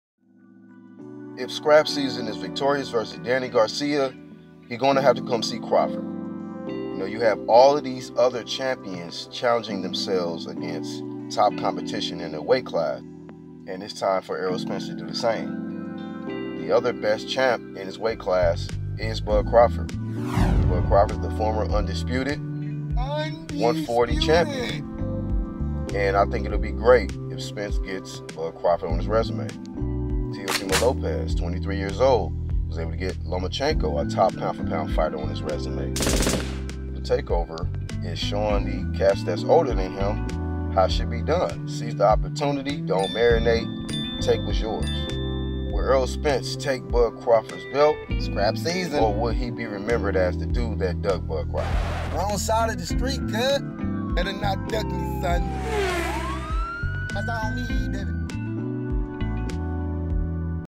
STRAP SEASON vs bud crawford.. when will it happen🔥?!
Errol Spence is gonna have to see Terence Crawford if he's victorious vs Danny Garcia. Otherwise he'll be remembered as the guy who ducked the former 140 pound UNDISPUTED champion.. period🔥!!